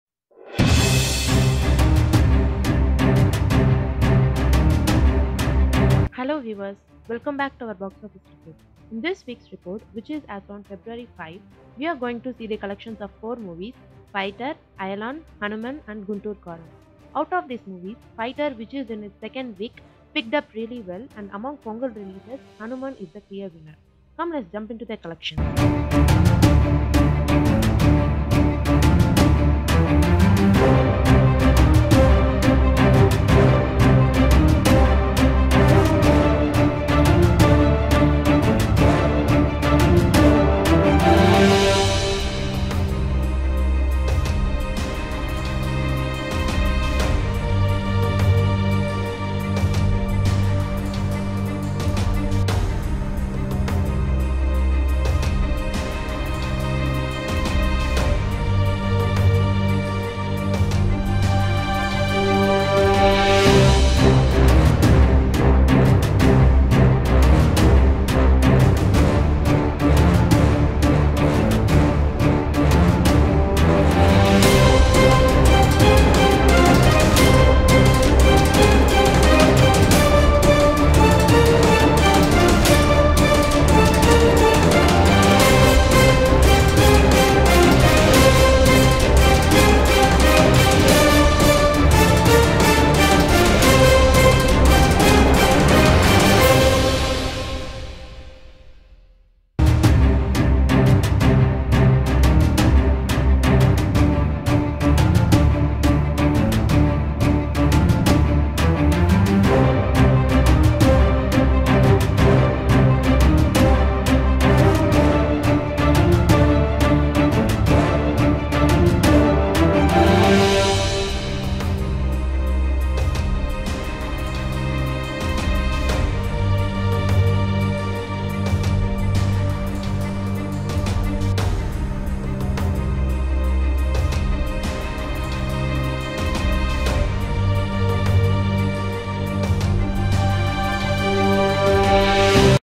Box Office Report: February 5 | Hanuman, Ayalaan, Fighter and Guntur Karaam Collections
In this video, we bring you the box office collection of Hanuman, Ayalaan, Fighter and Guntur Karaam Collections as of January 29.

Chapters:
0:00 Intro to our channel
0:06 About the Topic
0:30 Ayalaan Collections
0:58 Guntur Karaam Collections
1:25 Hanuman Collections
1:53 Fighter Collections